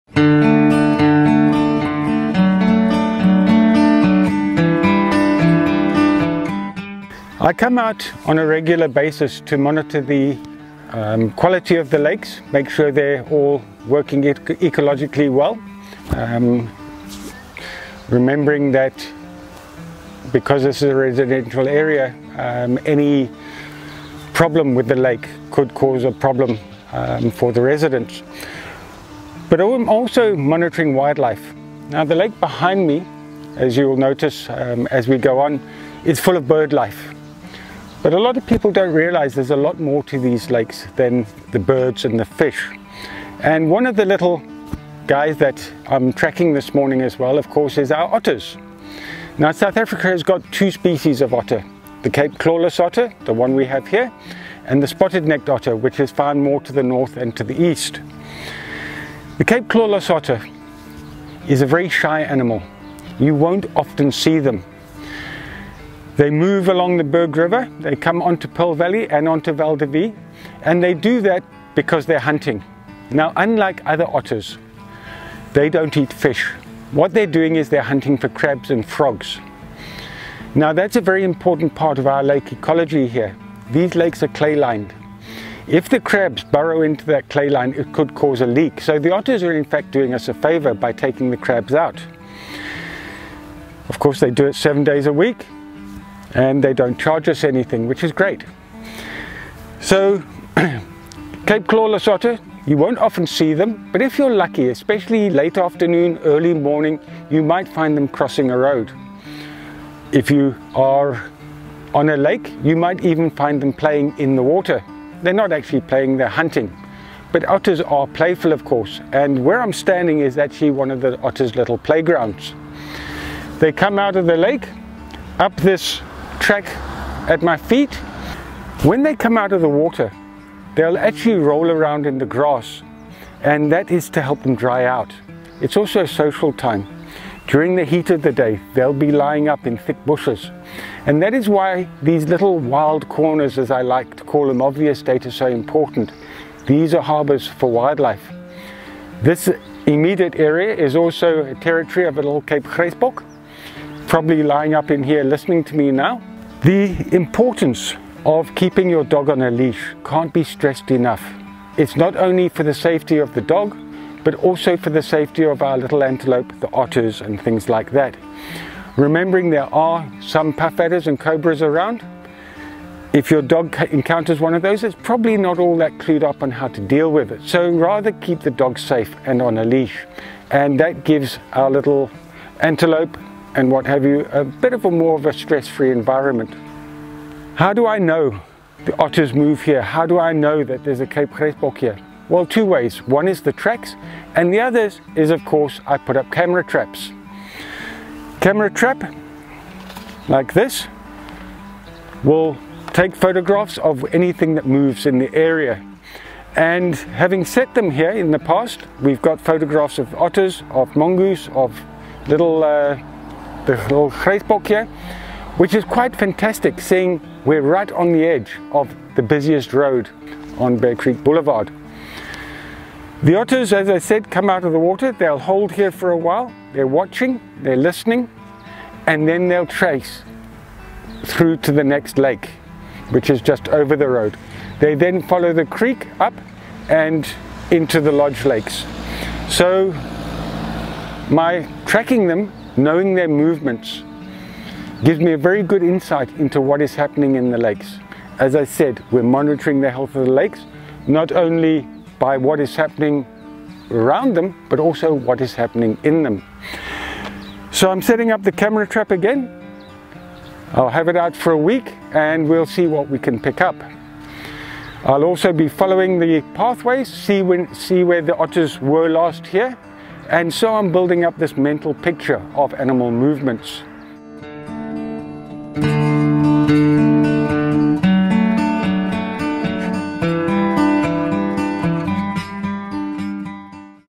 Life in the twilight zone | Environmental Projects on Val de Vie Estate
It’s hard to imagine that the 20 lakes and numerous large waterways on the estate were once lifeless sand quarries.  Today, a multitude of ecosystems flourish in and around these freshwater habitats.

Strategically placed motion sensor cameras give us surprising glimpses into the behaviour and habitat of a number of species, in particular, one very shy ‘acrobat’. Count yourself lucky if you see a Cape clawless otter moving from one lake to the other as they hunt for crabs, fish, frogs and worms. These crepuscular creatures are up before sunrise and asleep by time darkness falls.

We share our environment with an abundance of indigenous wildlife. We are their guardians of Val de Vie Estate and we urge residents to keep their dogs on a leash at all times when out and about.

Afrikaans name: Groototter
Xhosa name: iNtini

If you enjoyed learning more about the Cape clawless otters and how the environmental team maintains lake health across the estate, please like this video and feel free to leave a comment if you have any questions.

For more information on Val de Vie Estate’s sustainability program,